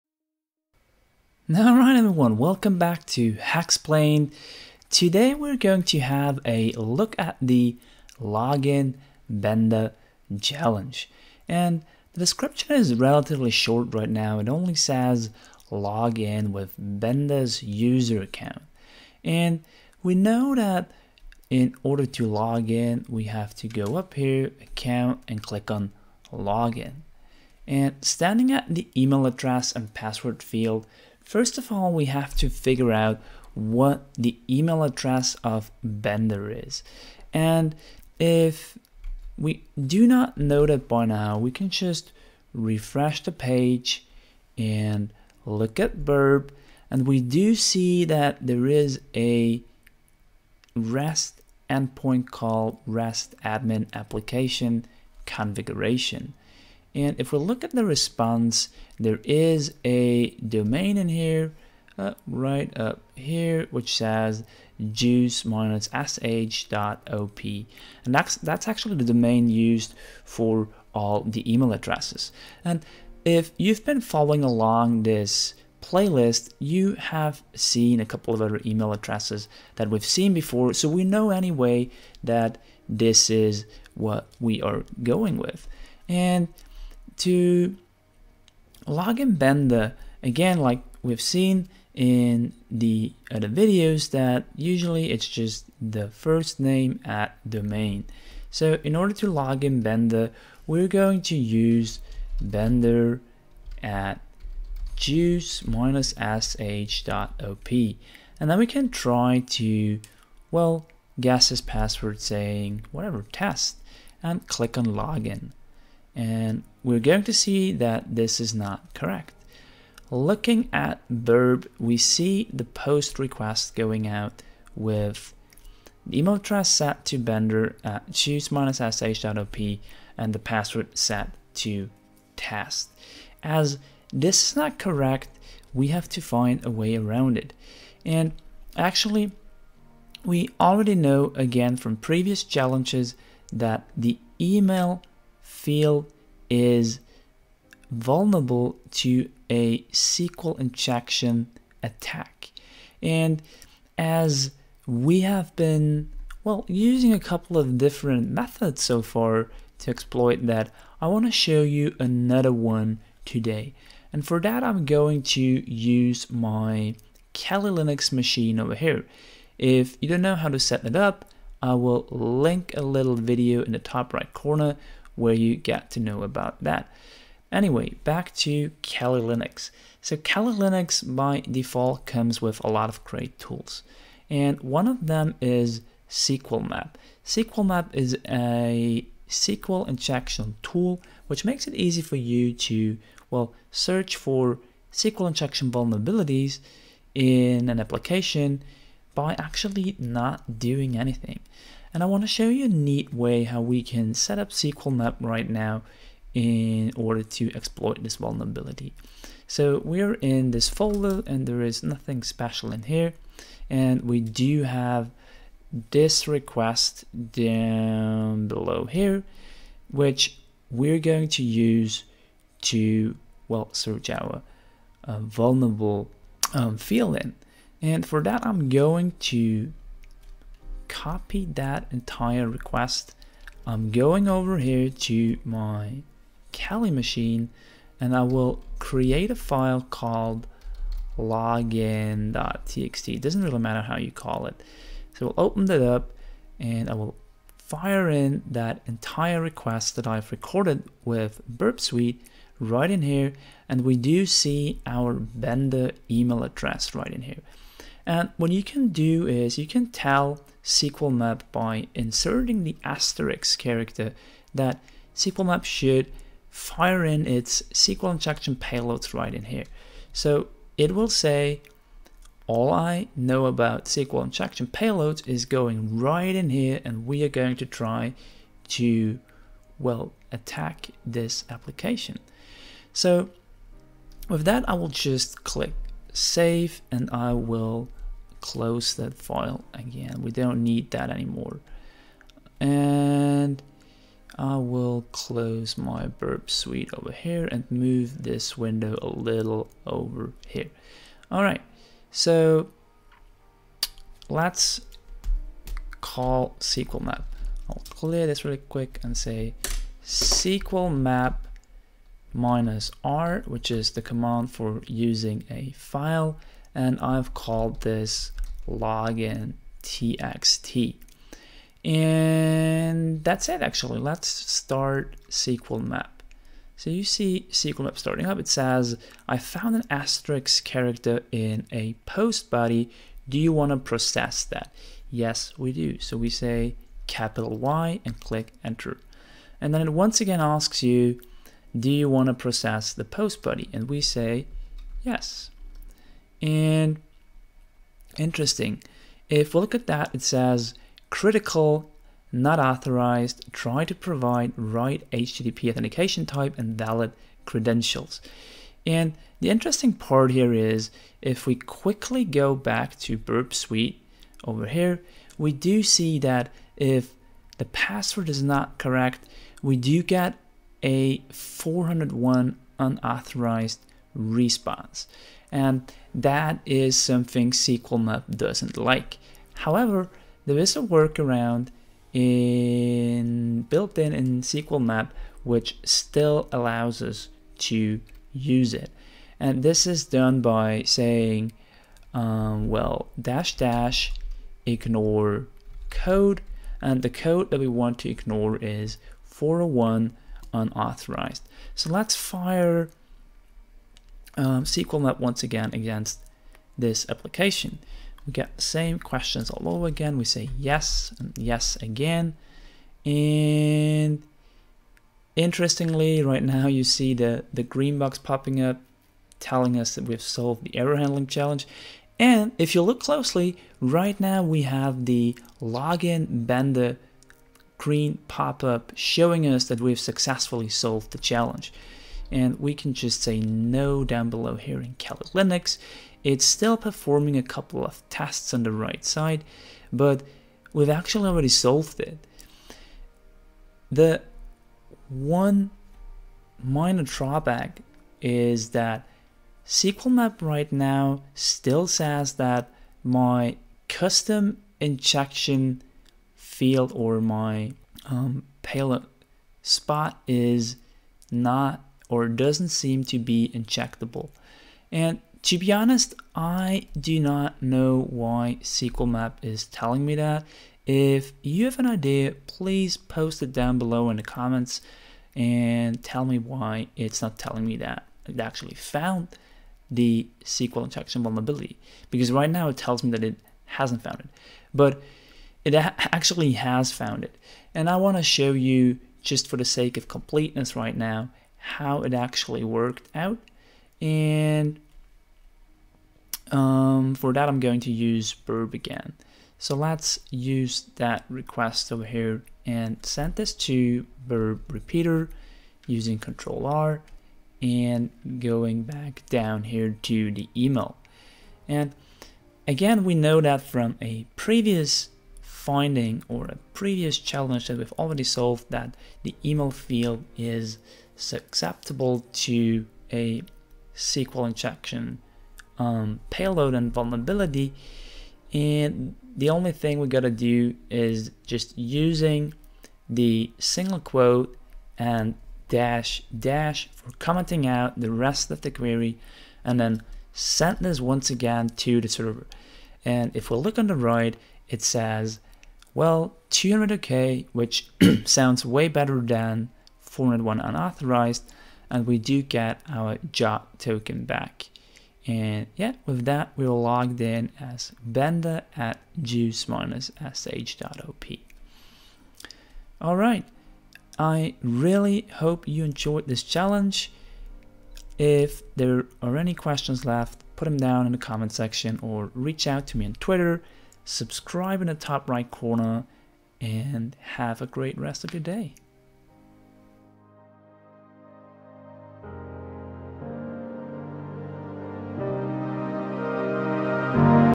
★ ★ ★ Login Bender (Injection)
Log in with Bender's user account.

🤴 Credits to Bjoern Kimminich for providing this excellent vulnerable web app.

💖 I need your help. This going to help make this project more sustainable in the long-run.